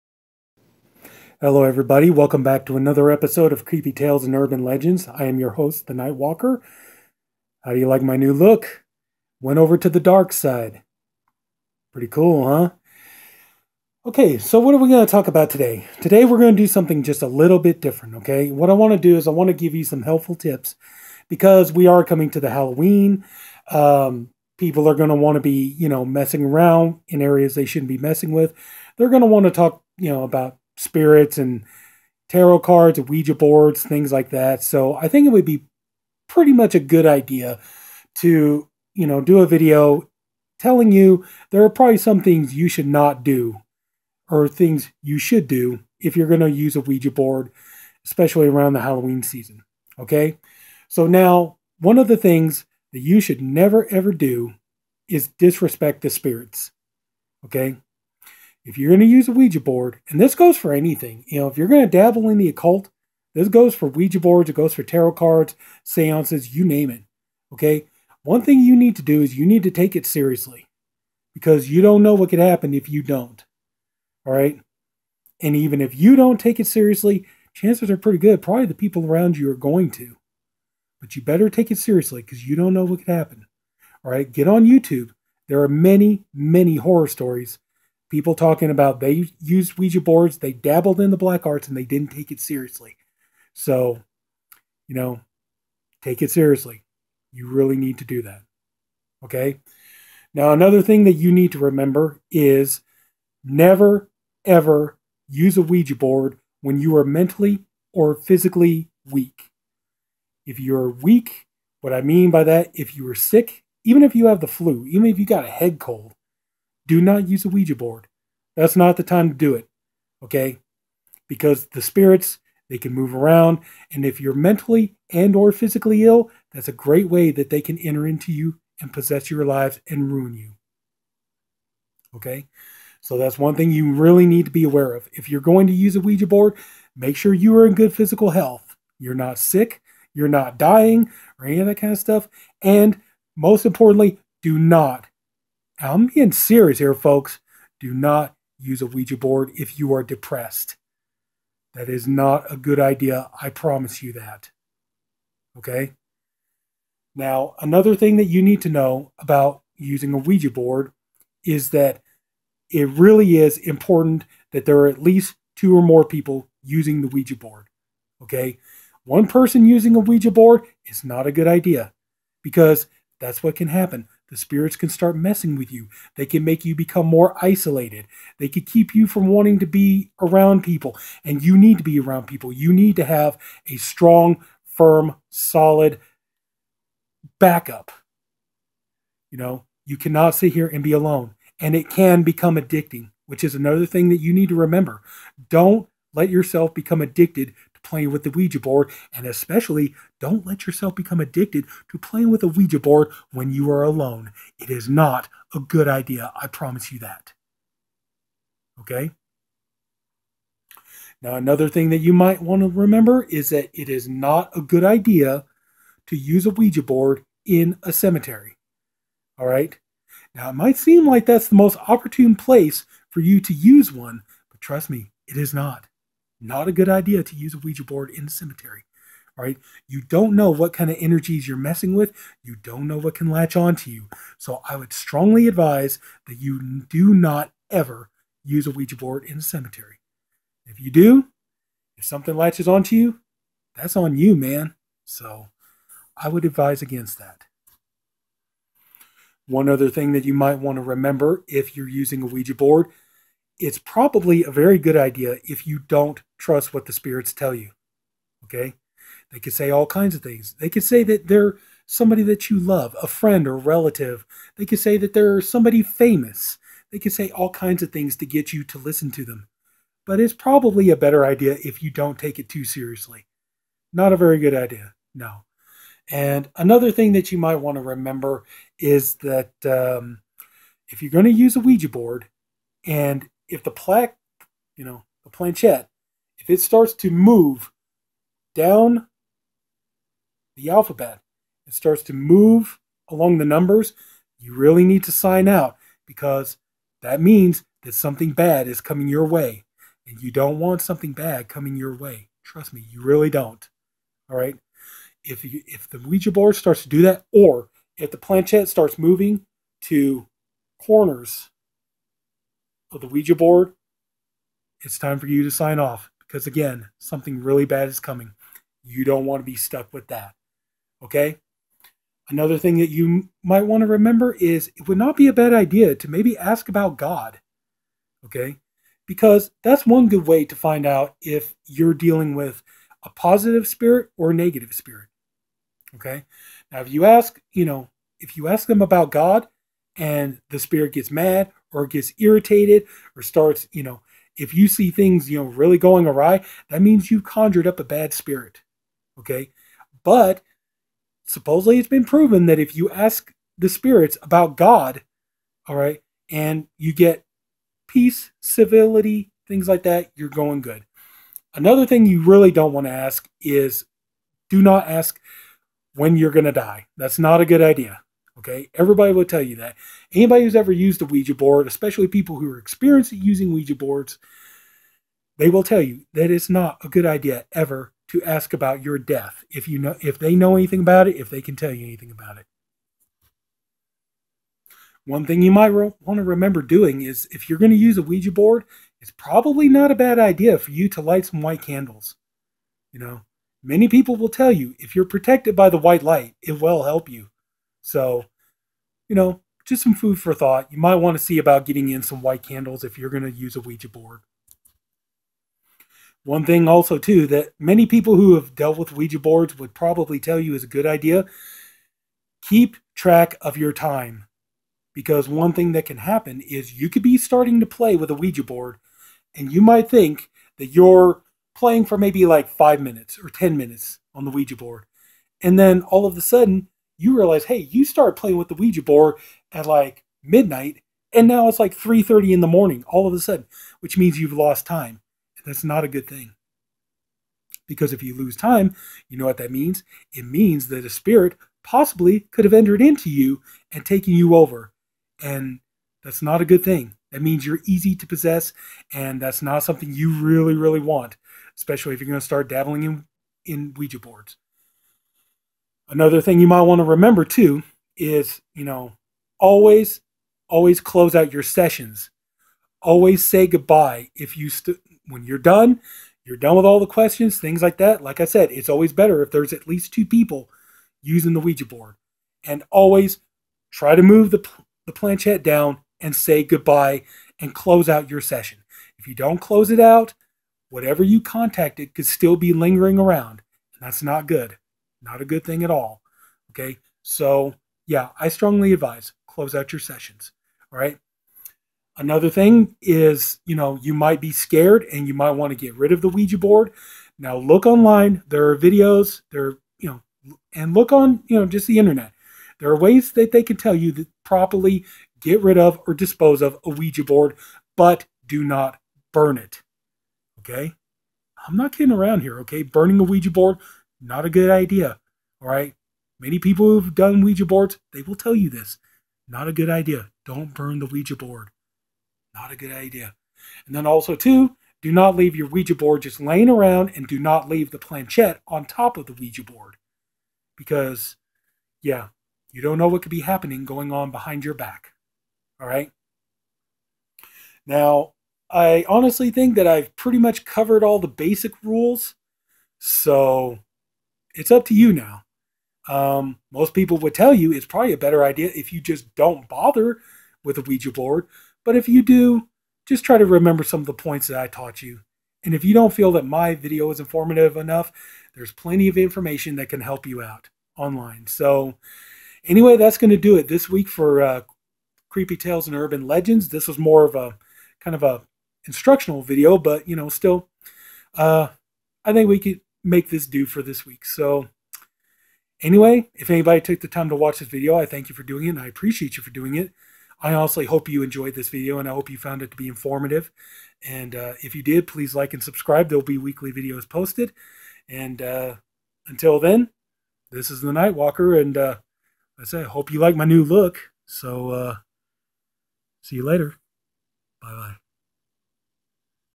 Creepy Tales & Urban Legends: Rules For Using A Ouija Board
Halloween is approaching and people want to contact spirits and use Ouija boards. Now, there are some rules one needs to know if they insist on dabbling with spirit boards. Let me give you some tips to (hopefully) stay safe contacting the spirits during the season.